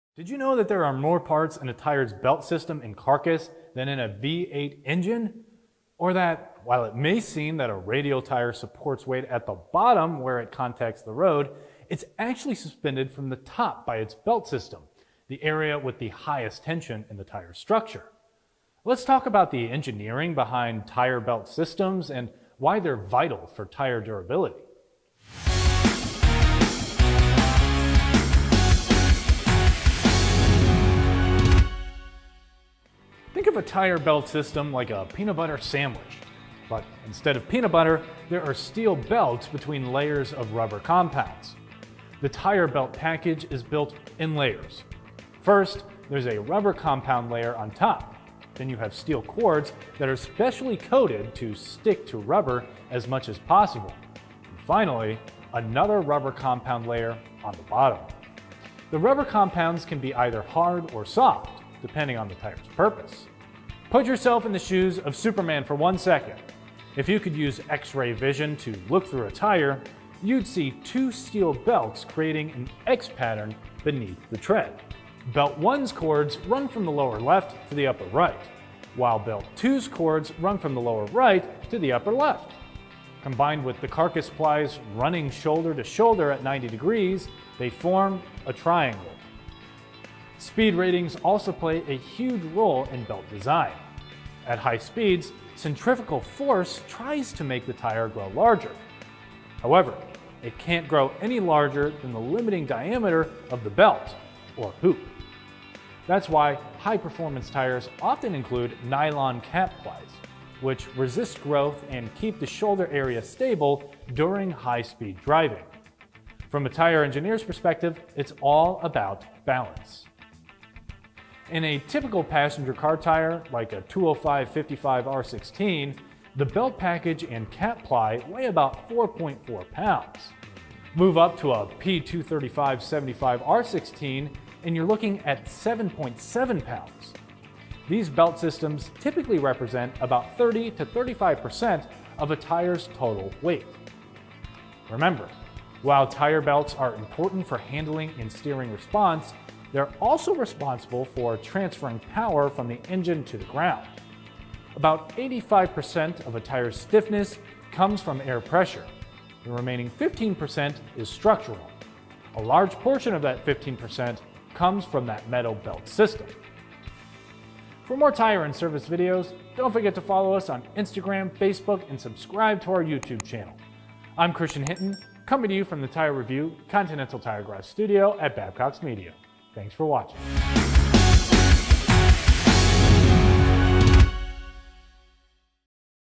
How a tire’s belt package affects durability
Did you know a tire belt system contains more components than a V-8 engine? Or that, while it may seem that a radial tire supports weight at the bottom where it contacts the road, it's actually suspended from the top by its belt system - the area with the highest tension in the tire's structure.

Let's talk about the engineering behind tire belt systems and why they're vital for tire durability.